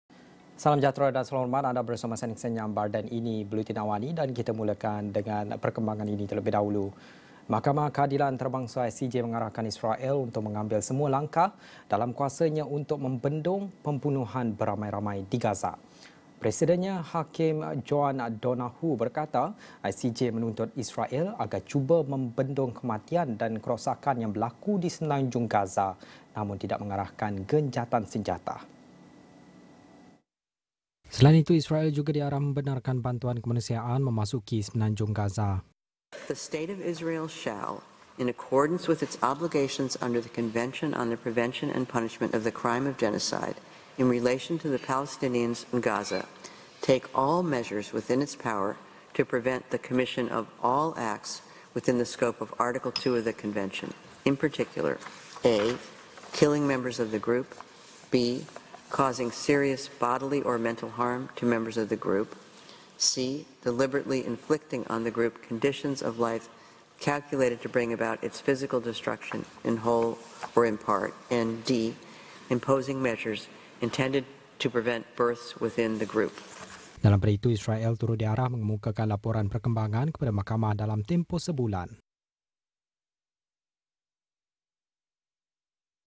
ICJ: Israel diarah bendung genosid di Gaza
Hakim Joan Donahue berkata ICJ menuntut Israel, antara lain, agar cuba membendung kematian dan kerosakan di Semenanjung Gaza, namun tidak mengarahkan gencatan senjata.